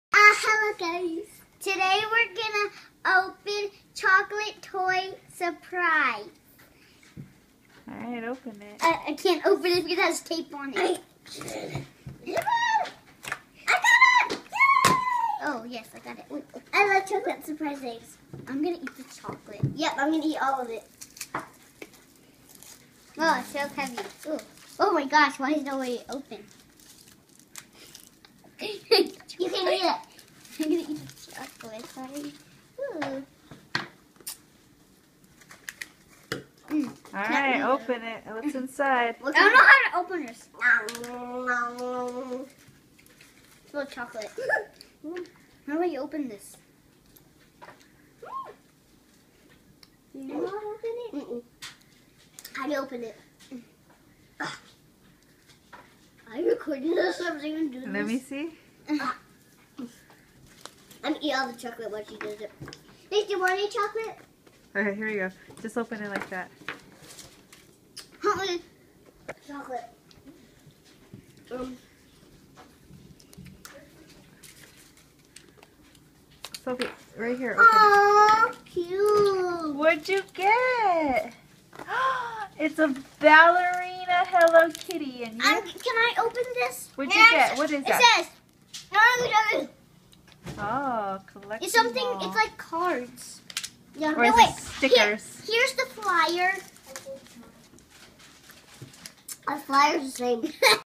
Sophie and evie open dollar tree hello kitty chocolate toy surprise eggs
Dollar tree surprise eggs